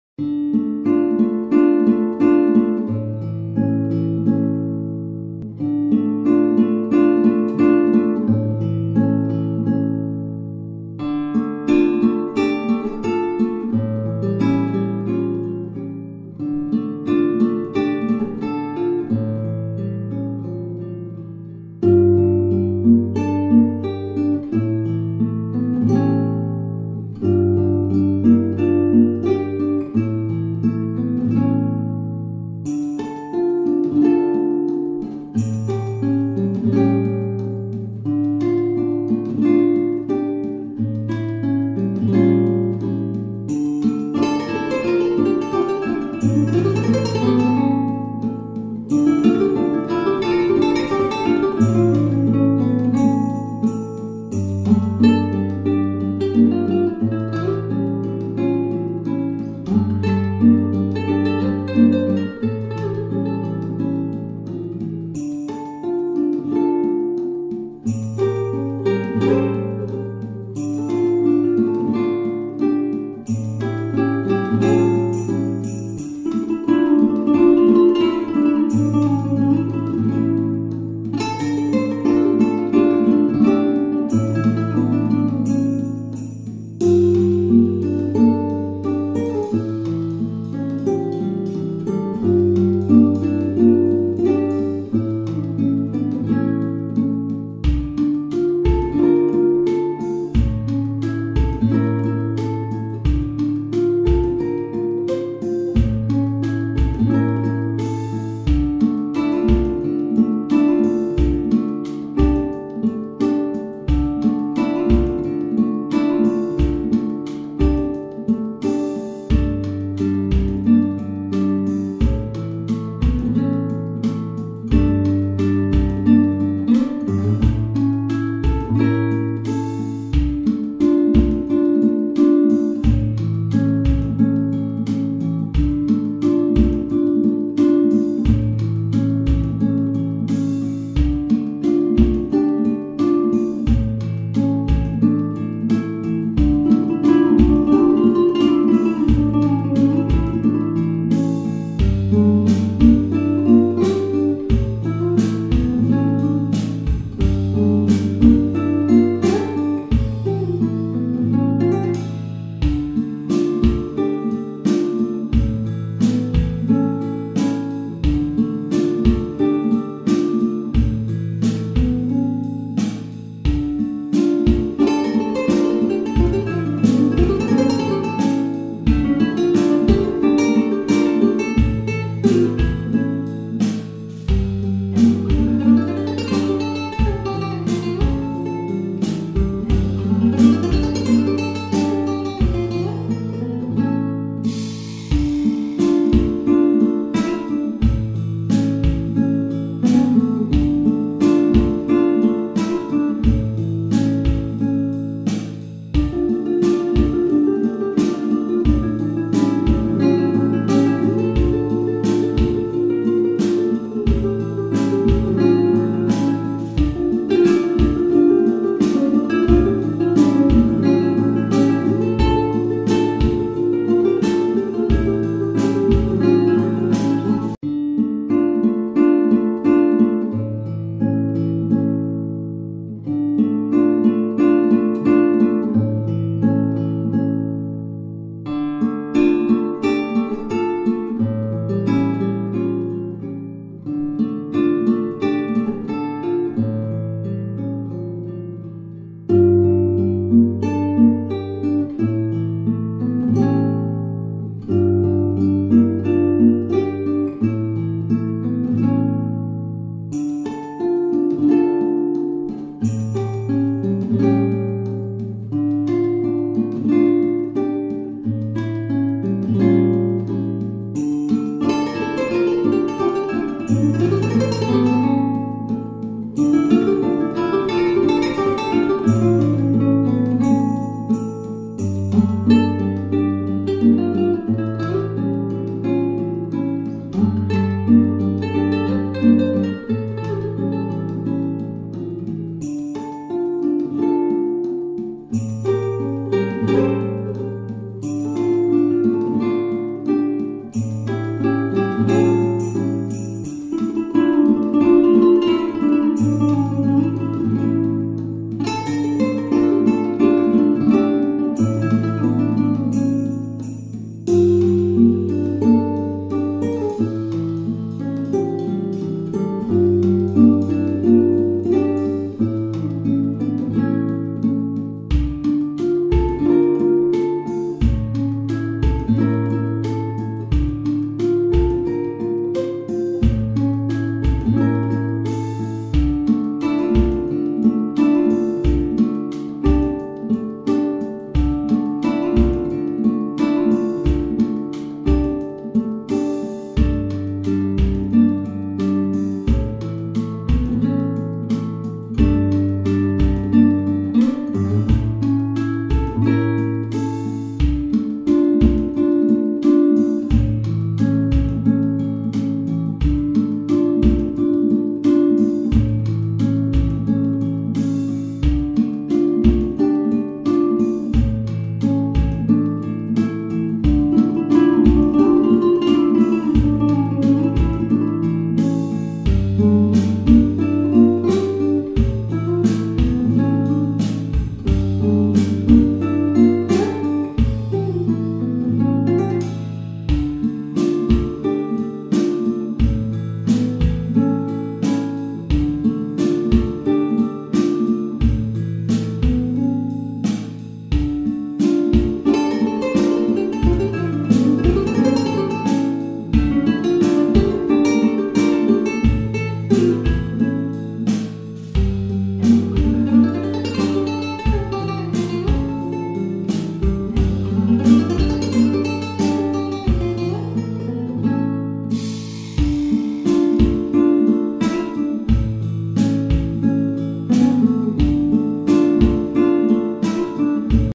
REALISTIC PORTRAIT DRAWING-time-lapse (BEGINNER)
A time-lapse drawing portrait using pencils size B-B6.